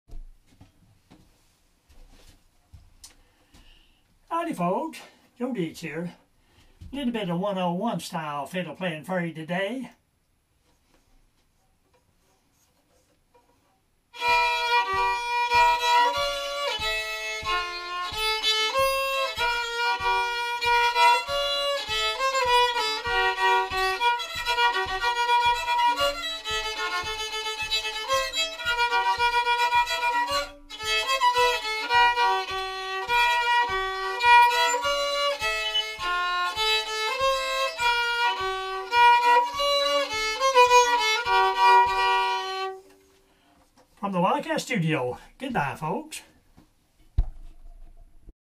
Wildcat Mountain Music #2,414
HELLO KIOWA COUNTY, KANSAS= Joe Deitz here with a bit of appalachian folk music.